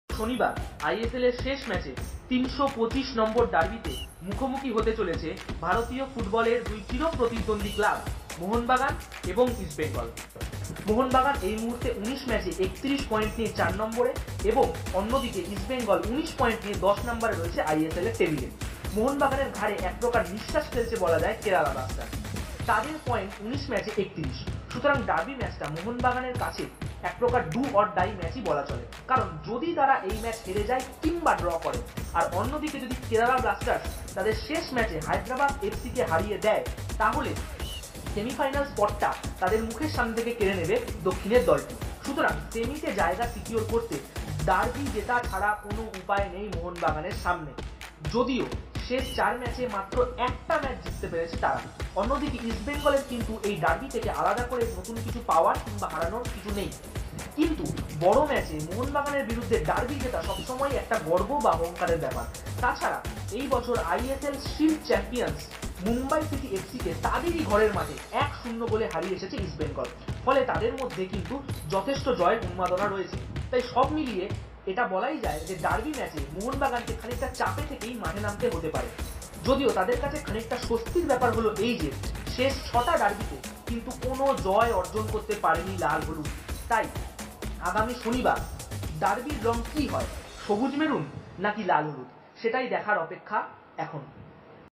ইস্টবেঙ্গল VS মোহনবাগান || KOLKATA DERBY || Match Preview
Match Preview of Asia's Oldest Football Derby

এশিয়ার প্রাচীনতম ফুটবল ডার্বির ম্যাচ প্রিভিউ